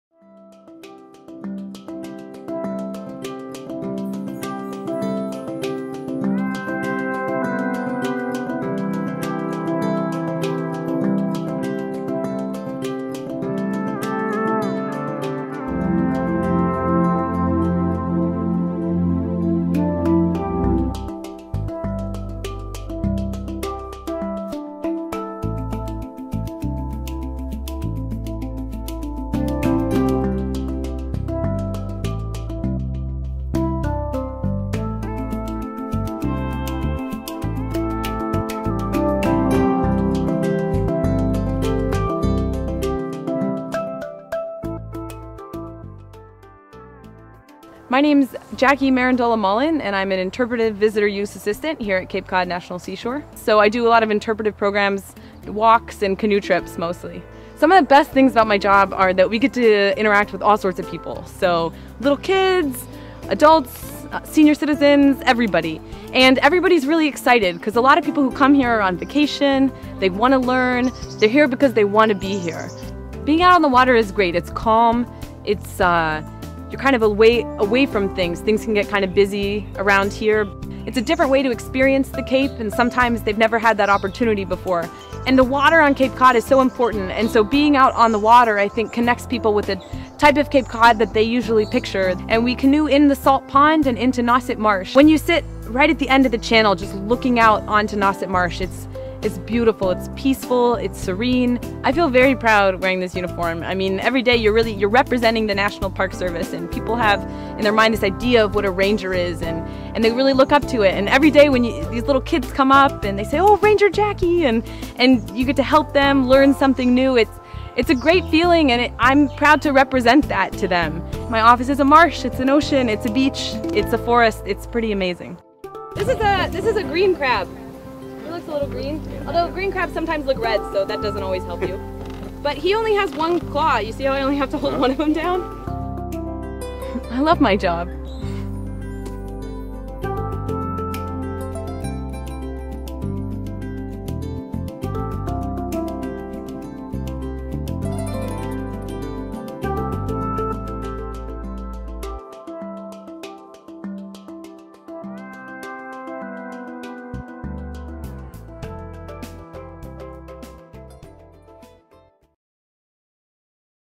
Profiles in the Park - Jackie Mirandola Mullen
Jackie Mirandola Mullen, Interpretive Visitor Use Assistant at Cape Cod National Seashore, takes a group of visitors on a canoe trip in Salt Pond and Nauset Marsh, and talks about her role as a National Park Ranger and the importance of water to Cape Cod.